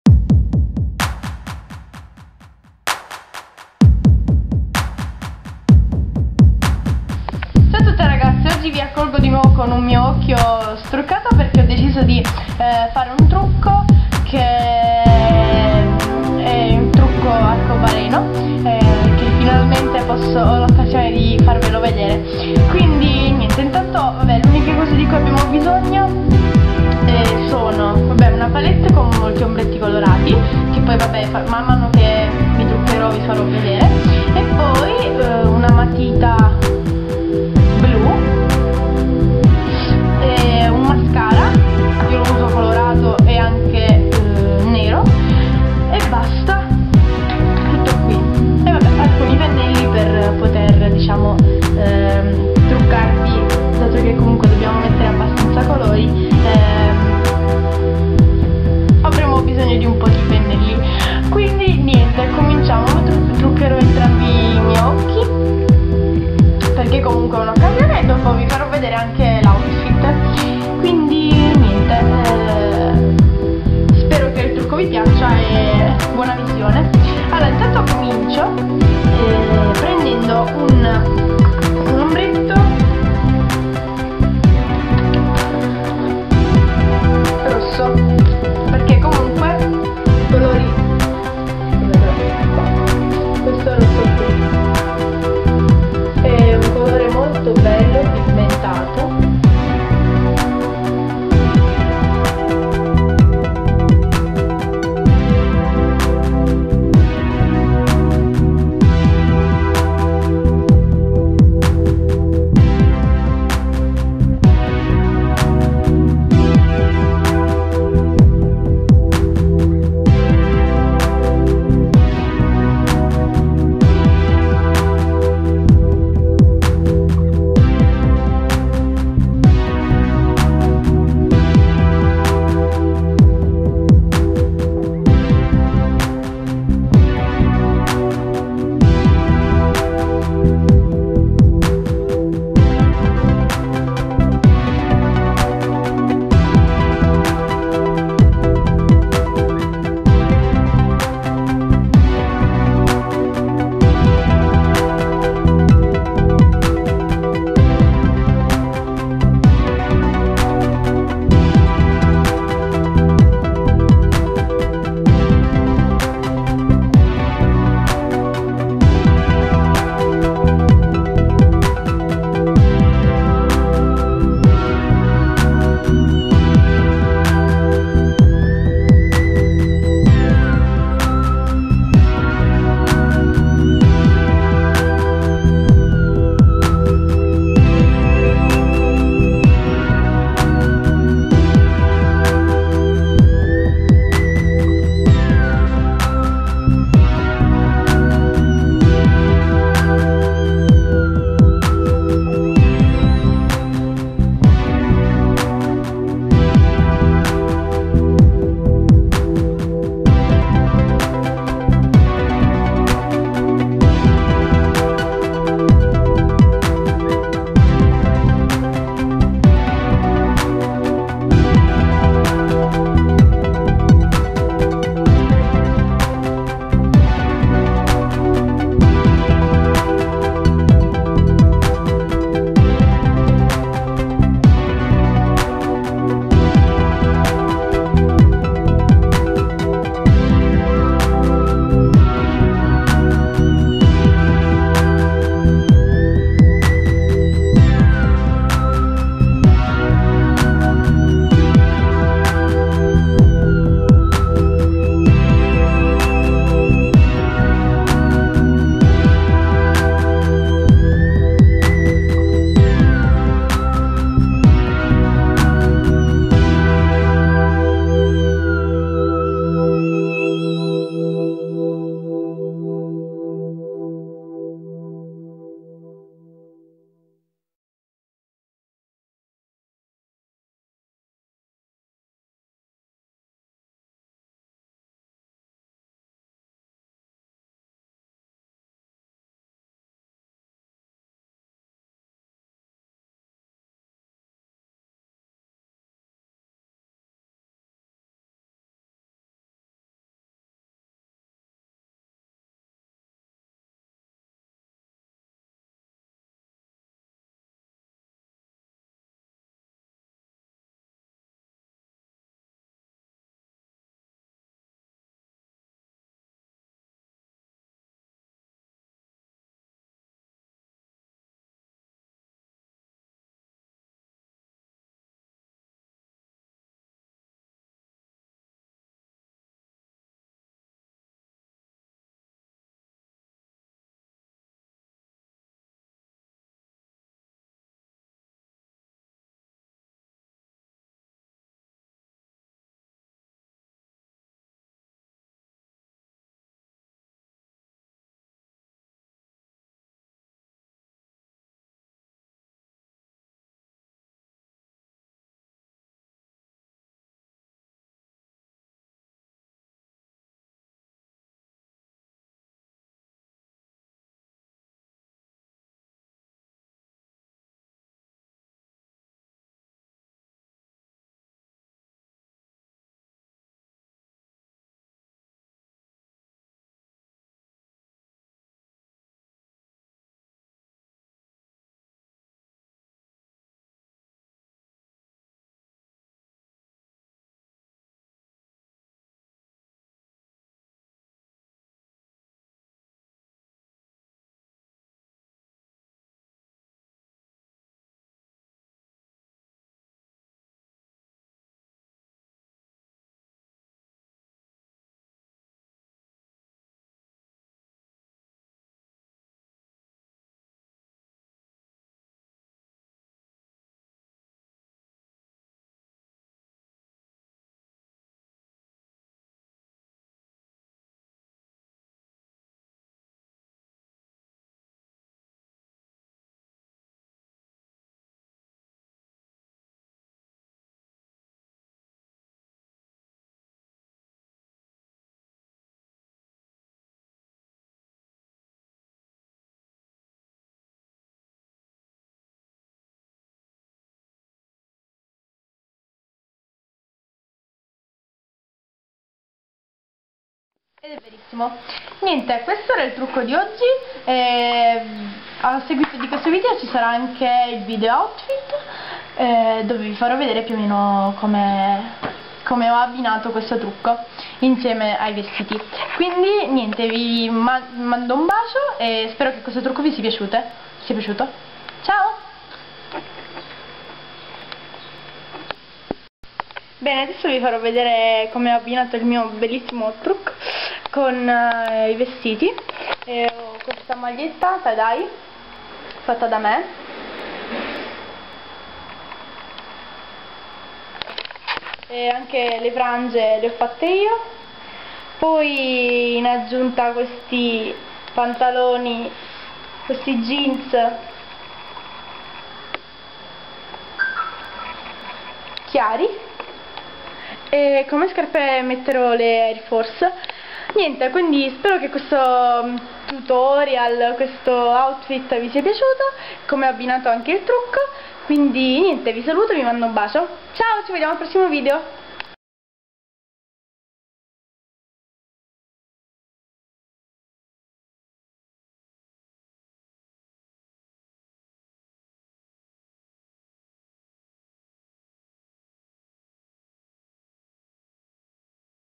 Rainbow Make up Tutorial and OUTFIT!!-Vanessa Malato-
Finalmente un trucco Colorato e Un video Outfit!
Segiutemi su instagram: vanessamalato98_MLOVE
E su Facebook! ♥